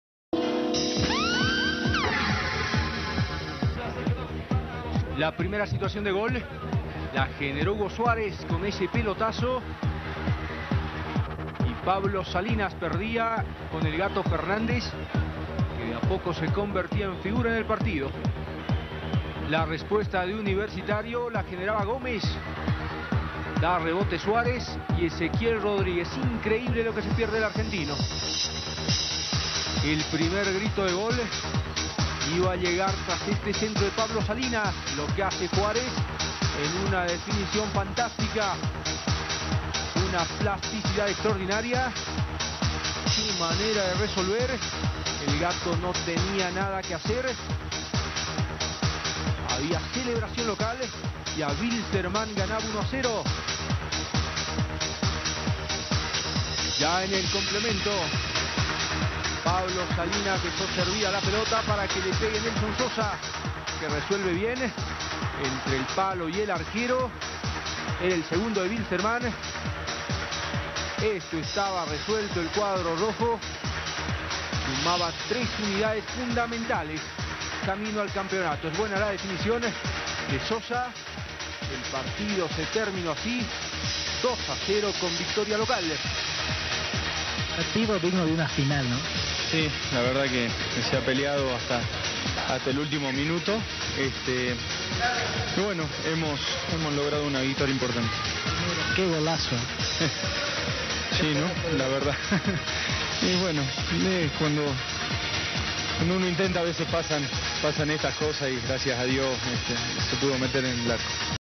Wilstermann 2 Universitario 0 (Hexagonal 2006)
El mejor gol del campeonato juares, campeonato clausura Wilstermann a un paso de ser campeon